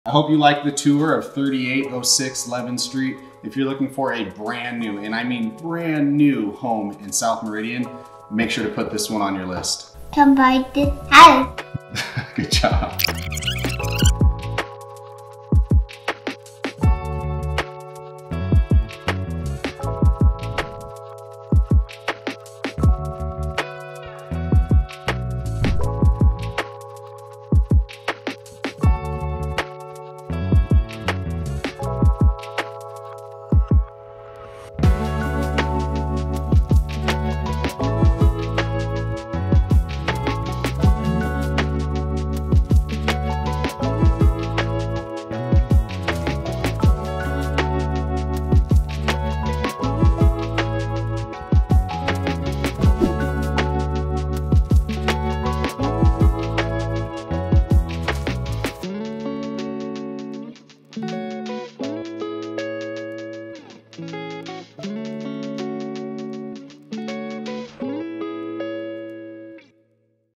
3806 E Levin St. Meridian, ID
Hurry, this one won't last! Brand NEW, never been lived in, single-level home located in the desirable sold-out Orchard Creek Community.

With over $30k in upgrades, this stunning home is ready for you to make your own! Spacious 4 bedroom, 2 bathroom layout. The kitchen features gorgeous modern finishes with stainless steel built-in oven/microwave and gas range/vent hood. Large island with plenty of room for breakfast bar seating. Enjoy backyard views from your kitchen sink. Master en suite complete with walk-in closet, separate shower and bath, dual vanities, and private access to the backyard.

Orchard Creek offers an ideal central location for a quick commute and easy access to both Meridian and Kuna, and downtown Boise within 20 minutes. Golfers will especially appreciate being close to Boise Ranch Golf Course. Venture out to the many green spaces, parks, and dog parks nearby.

In addition, there are plenty of dining, shopping, and entertainment options close by too. Don't miss out on this opportunity!

Whether you're looking for your first home, next home, or investing in the future, we are here!